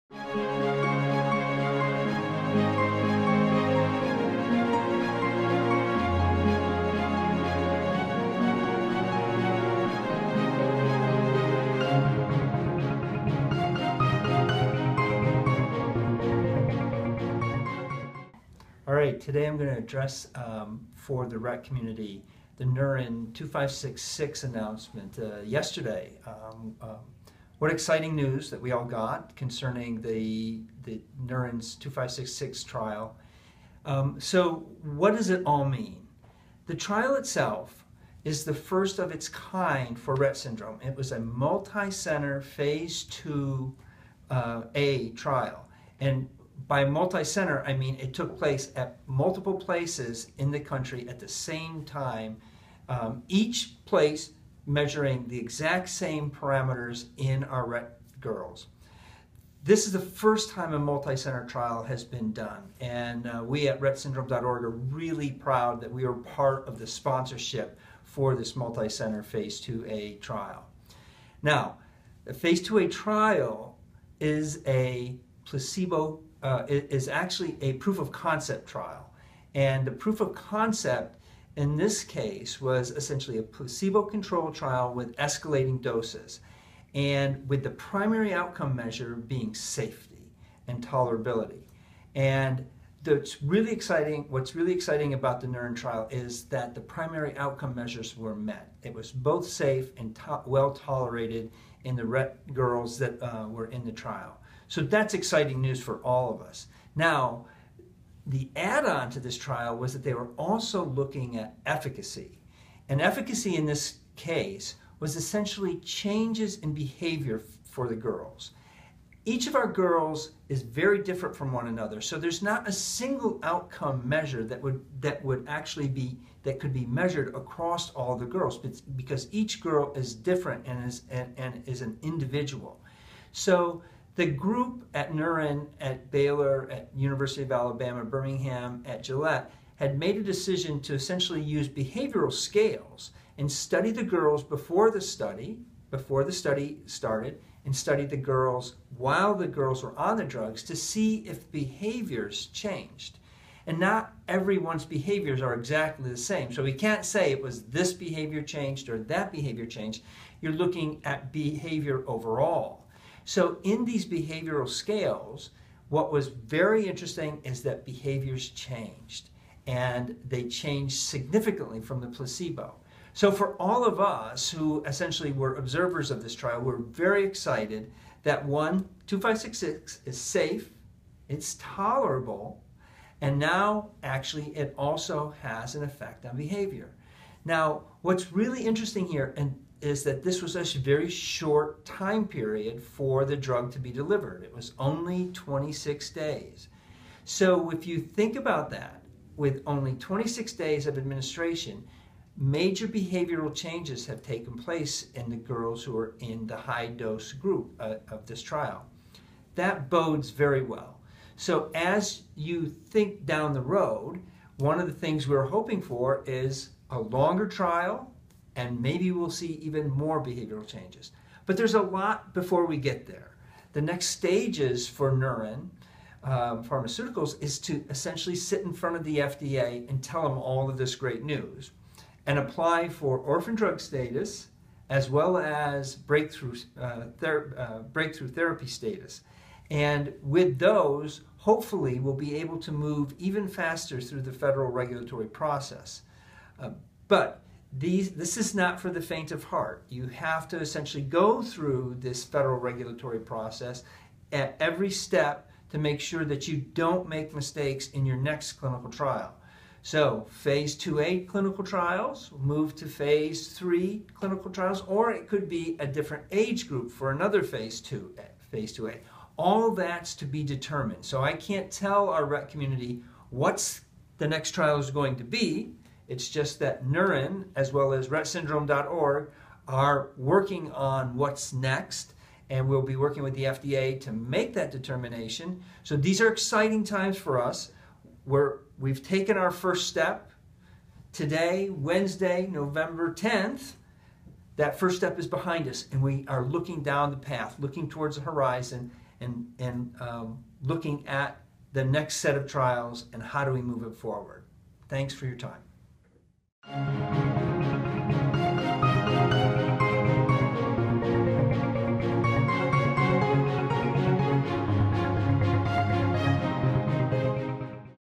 Rettsyndrome.org CSO Steve Kaminsky discusses promising Neuren drug trial results for Rett syndrome
Neuren Pharmaceuticals’ human clinical trial using NNZ-2566 as a potential treatment for Rett syndrome (a trial supported by Rettsyndrome.org) was completed in September of this year. Neuren has announced the results of this phase 2 placebo-controlled study, which investigated the dosing of NNZ-2566 for safety and tolerability with efficacy being studied as well.  The study was carried out at Baylor College of Medicine (Drs. Daniel Glaze and Jeffery Neul), University of Alabama Birmingham (Dr. Alan Percy) and the Gillette Specialty Healthcare (Drs. Tim Feyma and Art Beisang).

NNZ-2566 was shown to be well tolerated at the dose levels tested after 28 days of treatment, and no significant safety concerns were identified.  The data indicated dose-response and improvement over time in the study.  The high dose used in this study showed benefit vs placebo both in the group-level analysis as well as in the individual subject analysis.  For the details of the results please refer to Neuren’s announcement.

This is the first step needed to develop a drug for treatment of Rett syndrome.  The next steps for Neuren are to meet with the FDA and to apply for Orphan Drug Status and Breakthrough Therapy Status. This is exciting for all of us involved with Rett syndrome.  At Rettsyndrome.org, we continue to support Neuren in their goal to develop NNZ-2566 as a drug designated to treat Rett syndrome.

PRESS RELEASE - Neuren Announces Results of the NNZ-2566 Clinical Trial for Rett Syndrome

BLOG - NNZ-2566 success: Breaking it down and answering one common question: “What is this drug supposed to do for girls and women with Rett syndrome?”